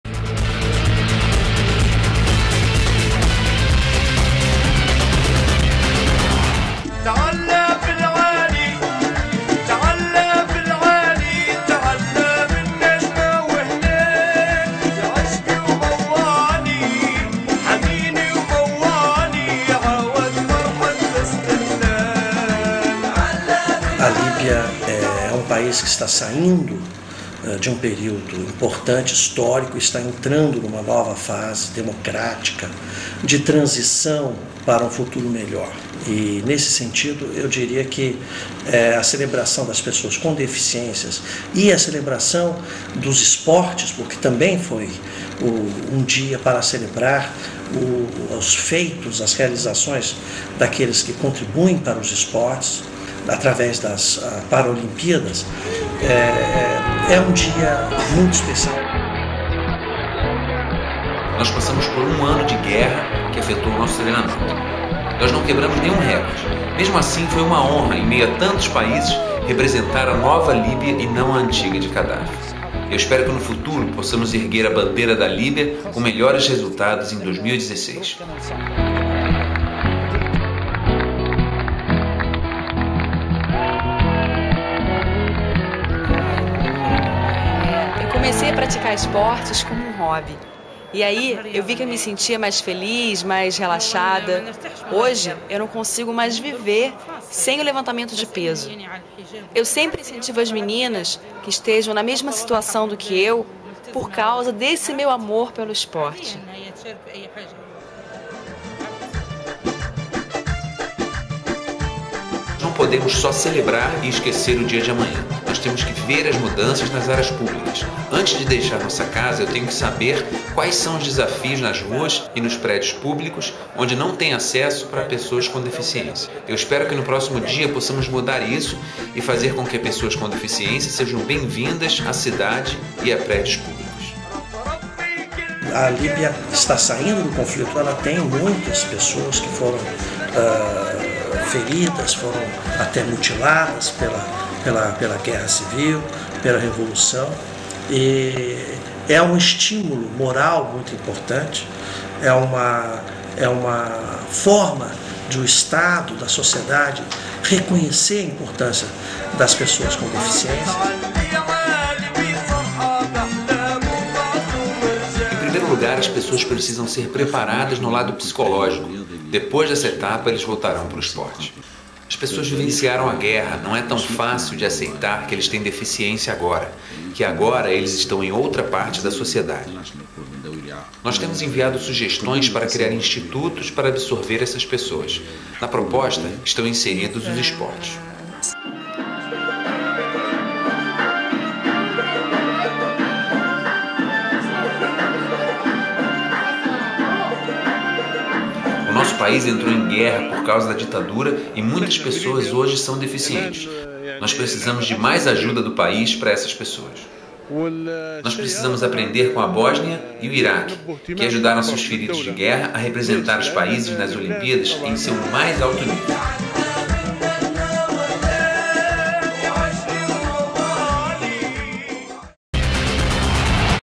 Ahe! visita a Líbia em homenagem às Pessoas com Deficiência
Ao derrubar obstáculos erguidos pelas diferenças, encontra-se um possível caminho para chegar à sociedade igualitária. Para aquelas em reconstrução, a inserção de diversos grupos funciona como condição para evolução. Provavelmente por isso, na Líbia pós-revolução, o Dia Internacional de Pessoas com Deficiência, proposto por um líbio - Monsour Al-Kikha - e criado há 20 anos pela Assembléia Geral da ONU, tenha um peso maior.